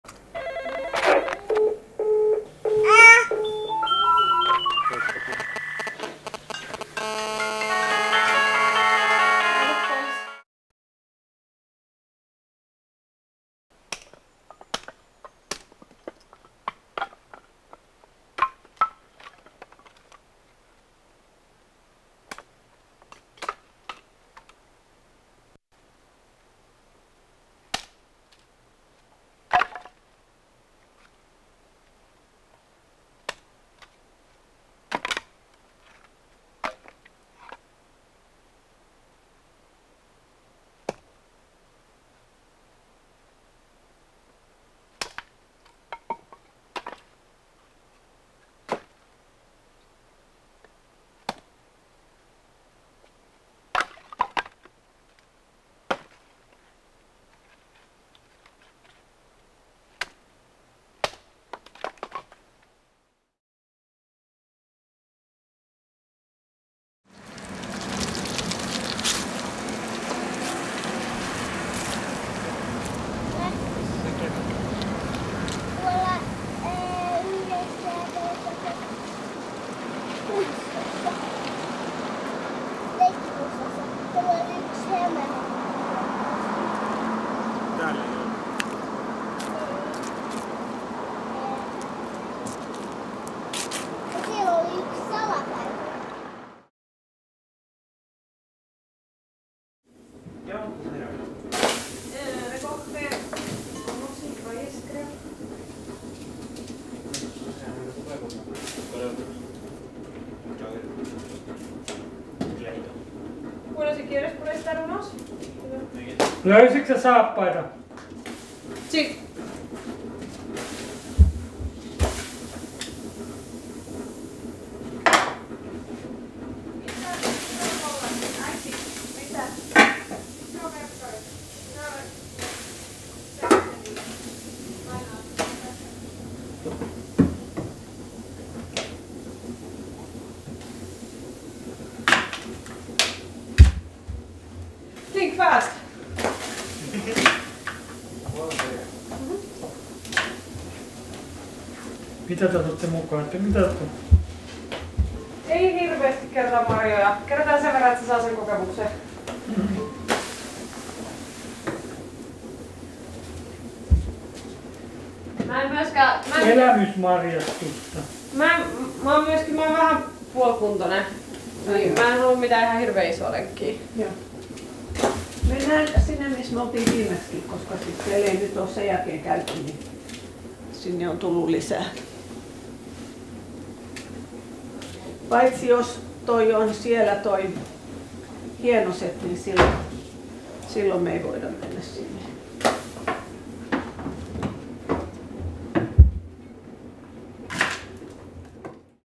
Episode 1654: Preparations for experience berry picking. September 20 / Year 4
Episode 1654: Preparations for experience berry picking

From three decades

Pekka K is a Finnish video artist who has kept a video diary daily since March 1990.